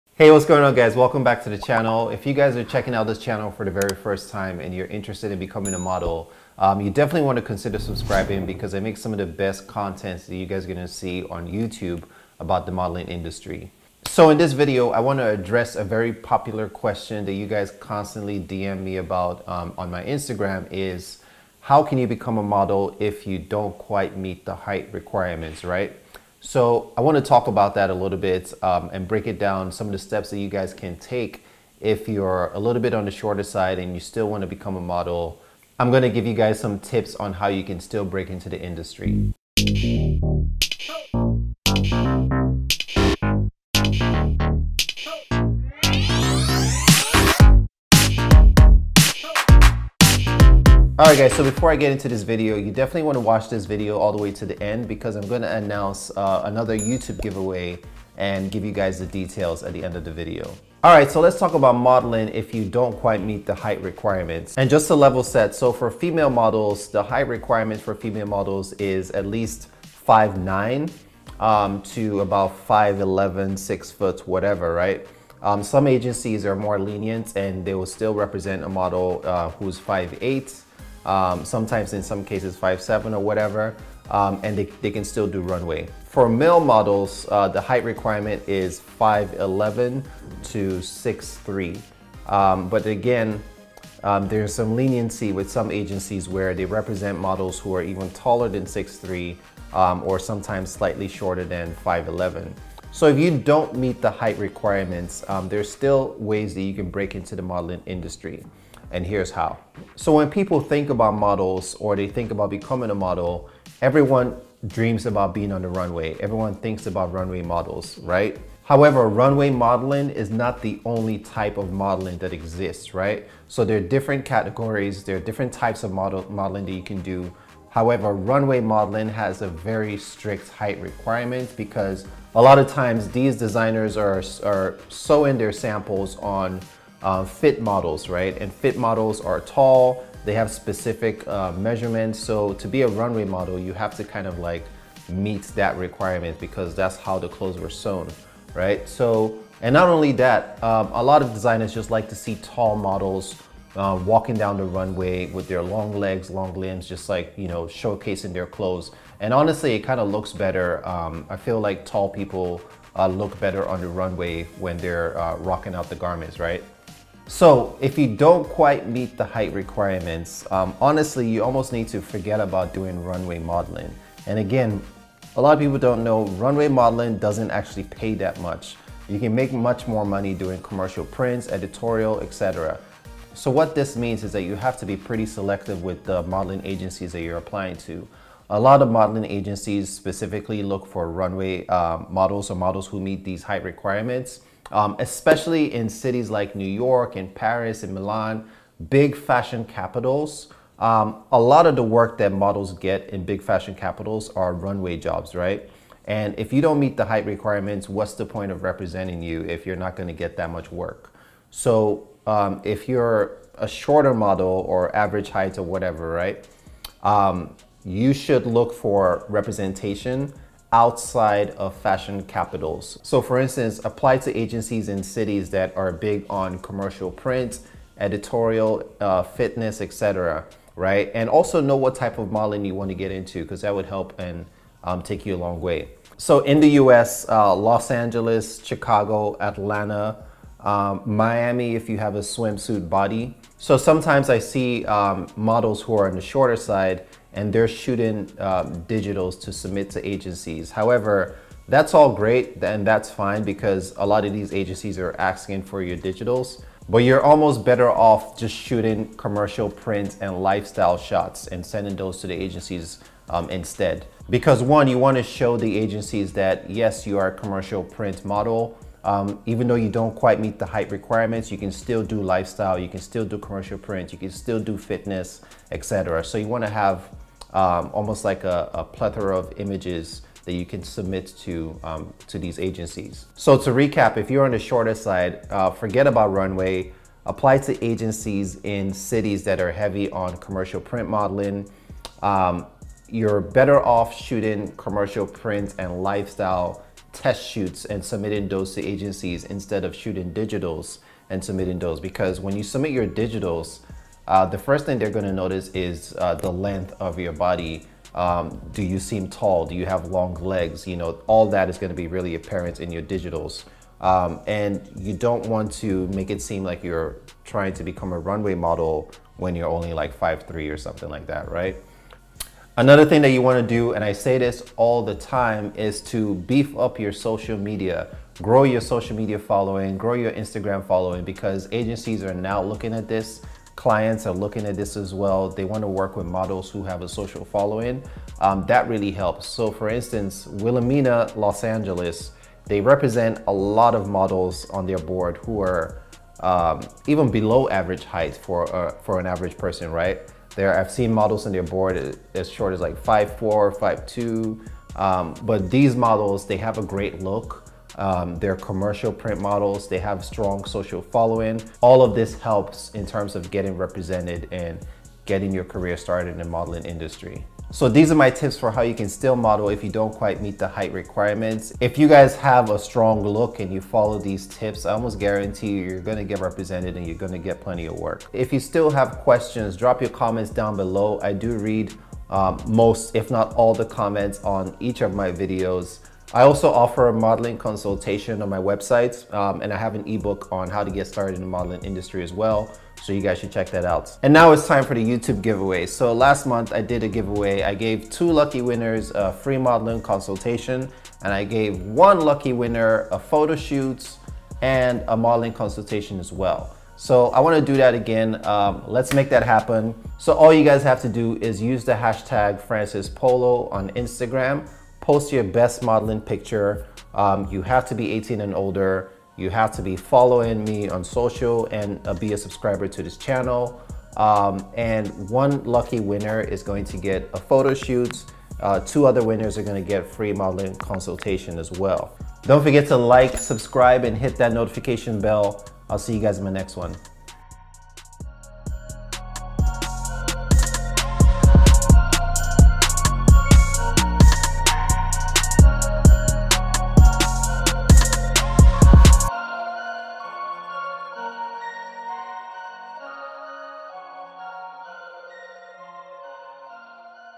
how to model if you are short
Not all models are tall, skinny, and statuesque. The height requirements for female models are between 5’9 - 5’11 and for male models it’s 5’11 - 6’3, however there are tons of models who fall below or above the height requirements and are still successful in the industry. Before jumping into the careers of the most successful short models, according to the fashion industry. Let's talk about pioneers like Kate Moss, Twiggy, Marilyn Monroe who were all undersized but were still able to have successful careers as models. Fall an inch or even half inch below the requirement and you'll be considered "short" even though you are taller than the average person. If you work where your strengths play out then you can be more successful.

In this video, I share some tips for how you can still be successful as a short model.

How to Start Modeling
One of the best ways to become a model is to start by attending agency open calls or doing an online submission as some of the models listed in this video did to begin their careers. Most of the Top Female Models in the world today are represented by IMG Models, DNA Models, Next Models, Society Model Management, and Elite Model Management.

Chapters
Intro - 0:00
YouTube Intro - 0:39
Annoucement - 0:50
Height Requirements for Models - 1:04
Modeling while short - 1:49
YouTube Giveaway - 6:33

Contest Details
*Must be 18yrs or older to win
*Use hashtag
*Winner announced on Jan 1st, 2022